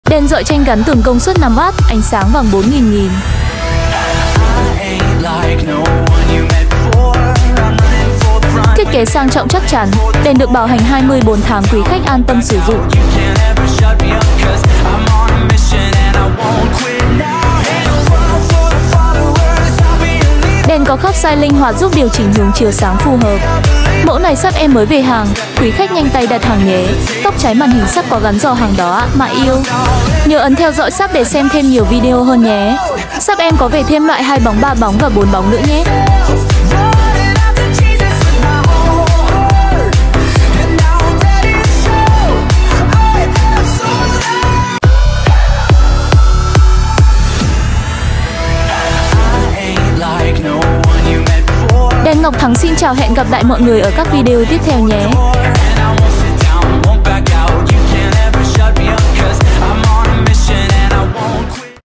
Đèn Led Rọi Tranh Trang Trí Phòng Ngủ Phòng Khách Led 5W 220V RT9118T1 Ngọc Thắng
Thể loại: Đèn LED rọi gương, rọi tranh, …
Kích thước: Phi 4.7cm x chiều dài ống rọi 11cm x đế 6cm
Công suất: Led 5W
Ánh sáng : Vàng trung tính 4000K
Loại bóng: bóng led tiết kiệm điện
Chất liệu: Hợp kim sơn tĩnh điện cao cấp
Màu khung: Màu trắng + inox
Nguồn điện: 220V
Sản phẩm : nhập khẩu
Bảo hành: 24 tháng
Ứng dụng: soi gương, bàn trang điểm, rọi tranh, ảnh, đồ vật,…